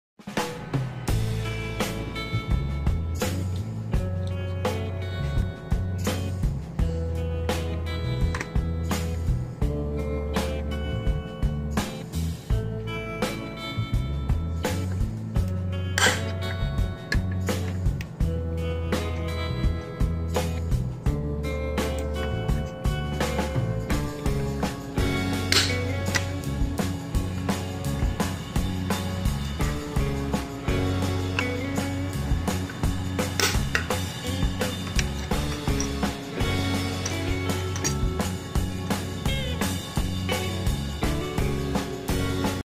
REGENI Pin puller for fixing pin of heat sink plate of TV PCB
To remove pin of heat sink plate of TV PCB